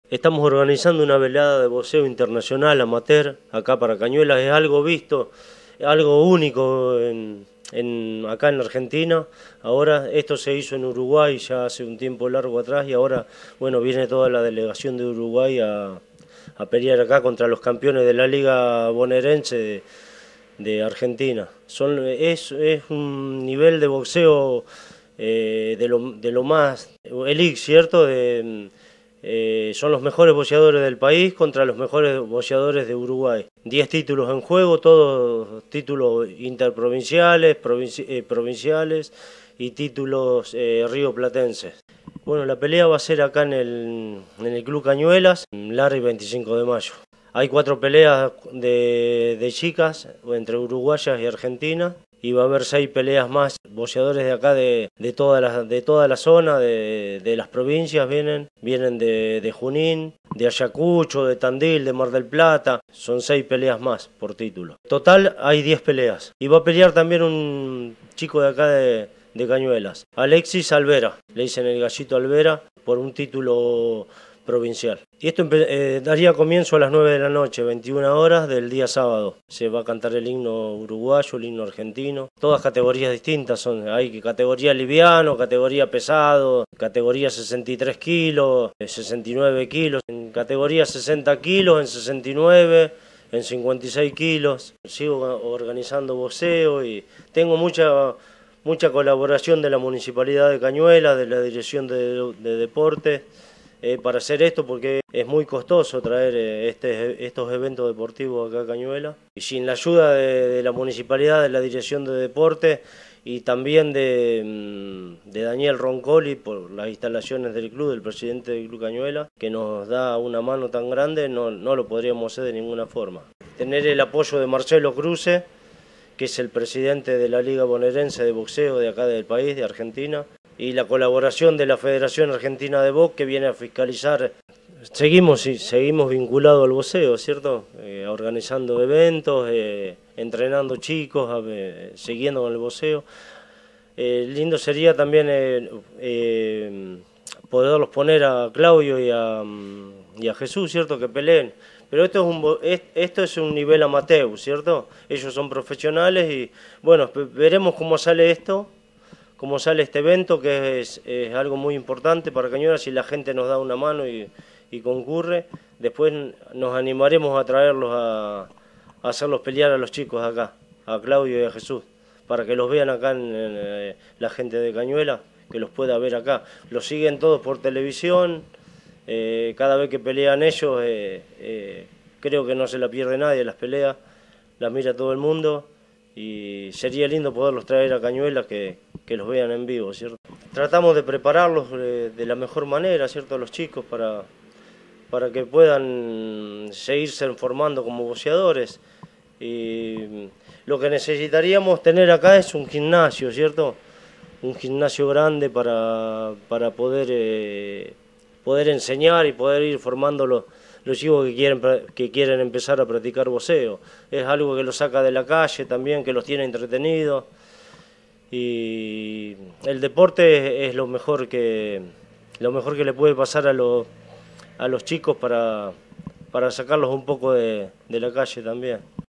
Vuelve el Boxeo a Cañuelas
Organizado por la Liga Bonaerense de Box Amateur, este sábado se pondrán en juego diez títulos Rioplatenses. También habrá cuatro coronas femeninas en disputa. Será en la sede del Cañuelas Fútbol Club, desde las 21hs. Hablamos con Adrián "Pingo" Daneff, uno de los impulsores de la velada.